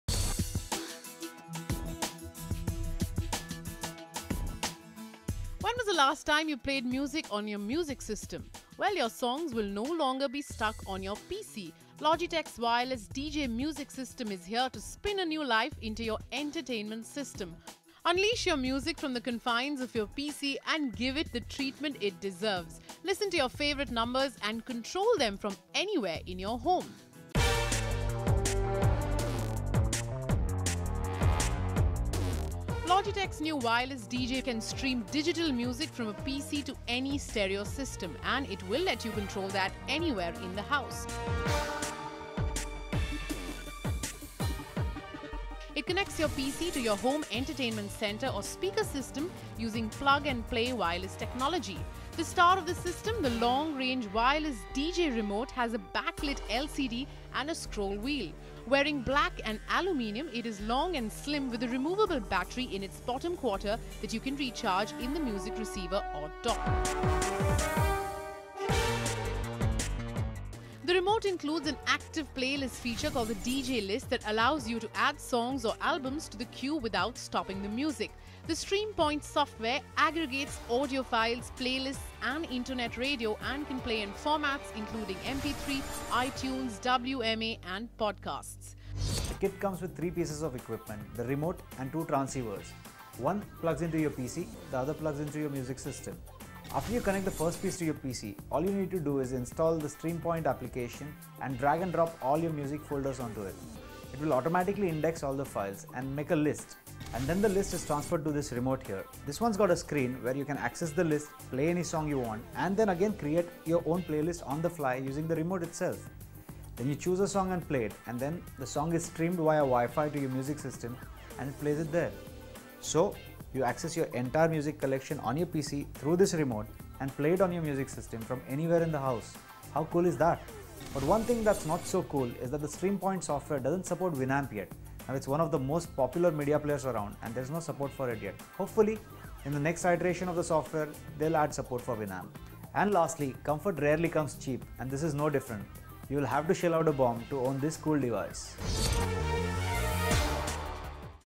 Logitech DJ
Logitech's Wireless DJ Music System is here to spin a new life into your entertainment system.